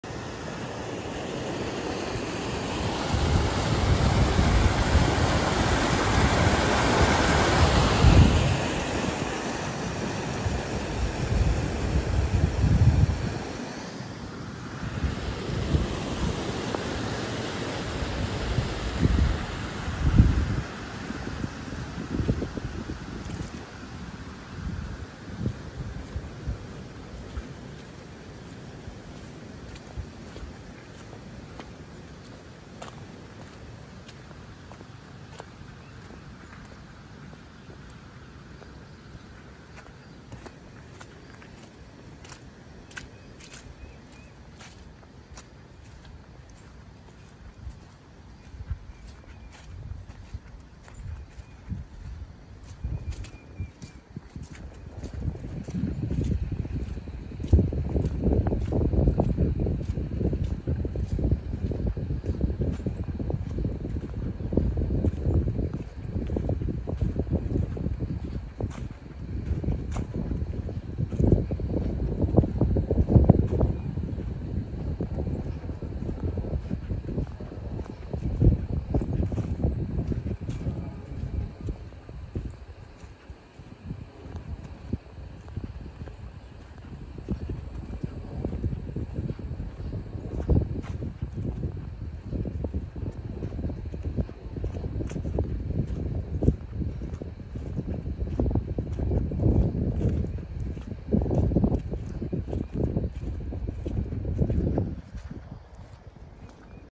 Update on the lake
The lake definitely got a lot lower it’s not on the dam anymore and it’s going down very slowly